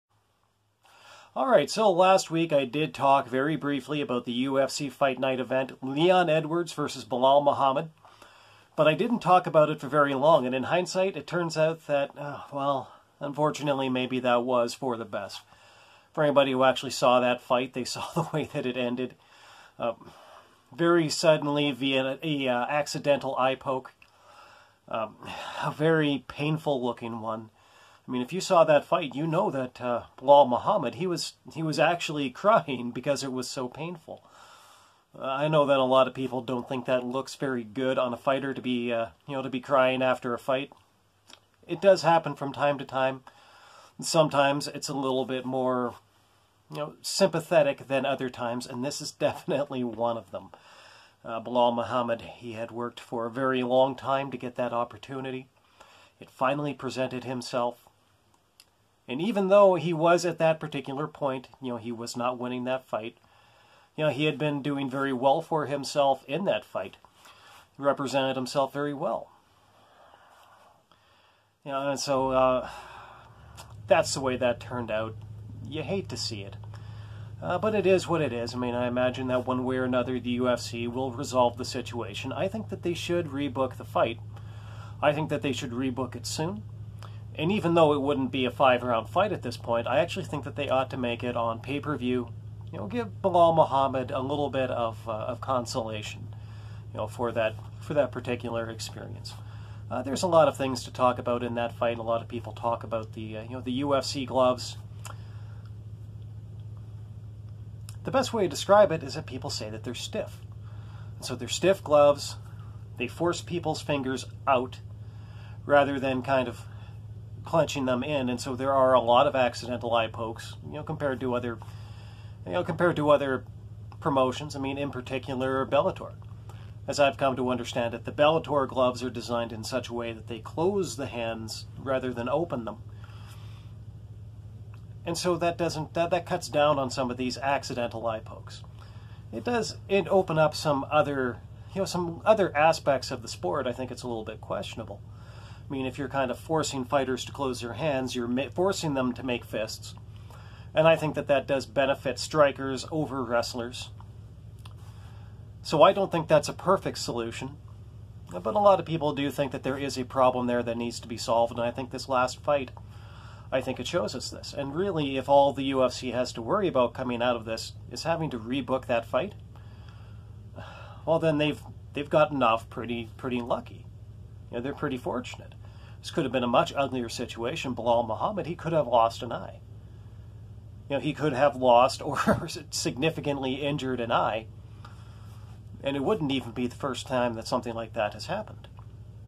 A Sad End for Edwards vs Muhammad
03/17/21 - Leon Edwards vs Belal Muhammad was not the exciting main event fight we were hoping for. But not because of anyone's fault.

Hopefully the UFC will reschedule the fight soon.